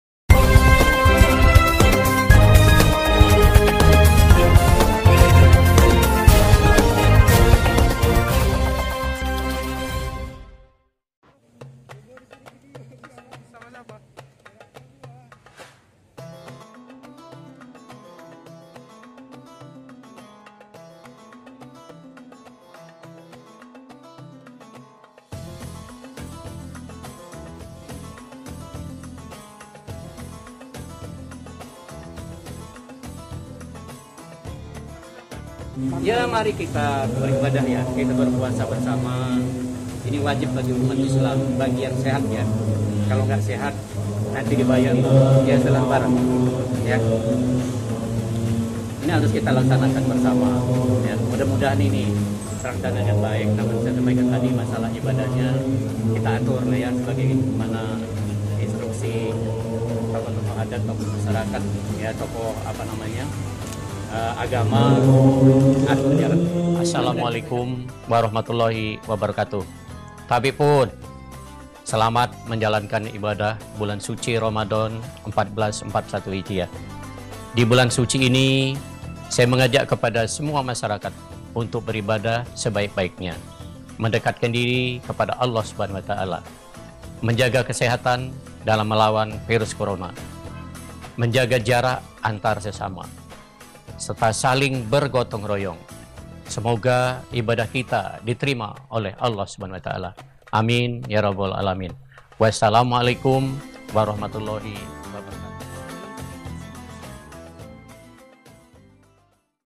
Ramadhan 1441 H, Dekatkan Diri Dengan Sang Pencipta dan Jaga Kesehatan
Bandarlampung : Ramadhan 1441 H yang jatuh di 23 April 2020 jadi lebih berbeda dengan adanya pandemi virus corona yang menyebar di seluruh dunia.

Memaknai Ramadhan kali ini Walikota Herman HN berpesan untuk warga kota Bandarlampung untuk selalu mendekatkan diri dengan sang Pencipta serta tetap menjaga kesehatan.

Ia juga mengingatkan kepada warga kota Bandarlampung untuk saling menjaga jarak dan bergotong royong dalam menghadapi pandemi virus corona.

Alvian Today TV Indonesia